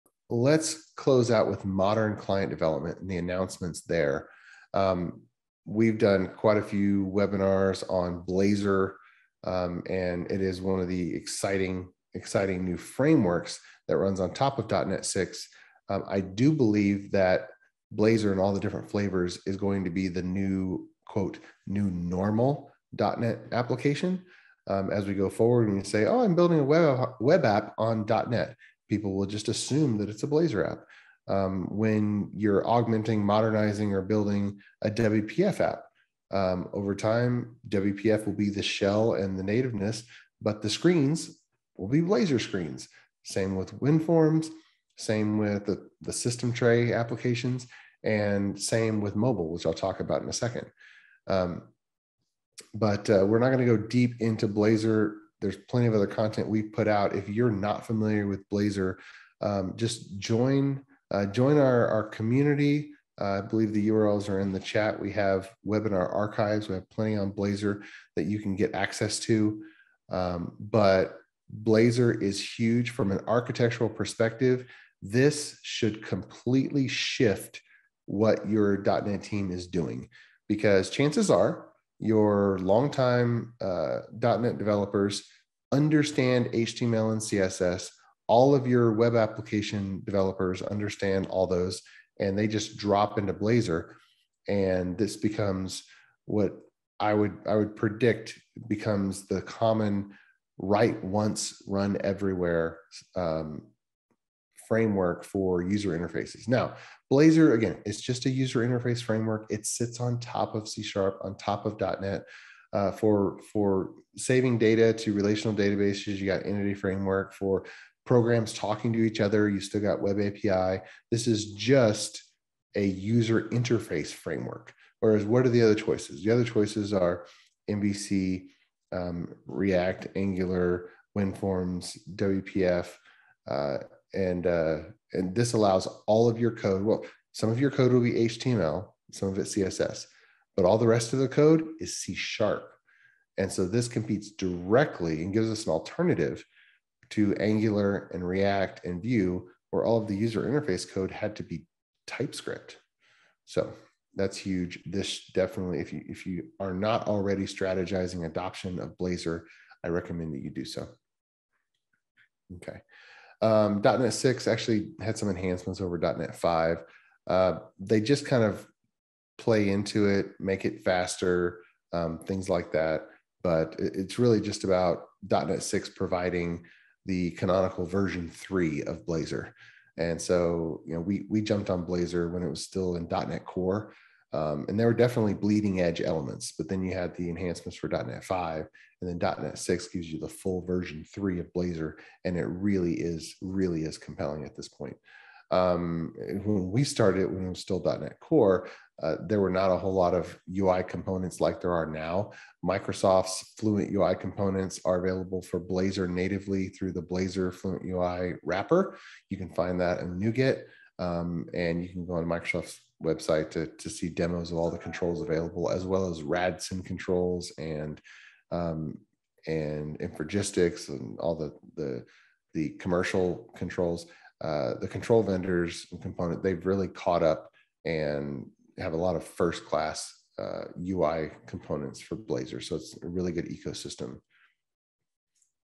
Blazor Fluent UI Components | Webinar Clips, .NET 6 & Visual Studio 2022
.NET 6 is here!  It is the highly anticipated long-term release from Microsoft aligned with a new version of the flagship IDE, Visual Studio 2022.

Within it is support for more client-hosted applications through Blazor as well as the upcoming MAUI mobile apps. C# 10 is a new version of the primary programming language, and the framework enhancements are too numerous to list.

With new capabilities and choices, your head may be swimming. Join us for a webinar to help you sort out how .NET 6 impacts your architecture decisions both in your software architecture as well as your DevOps environments and production environments in Azure.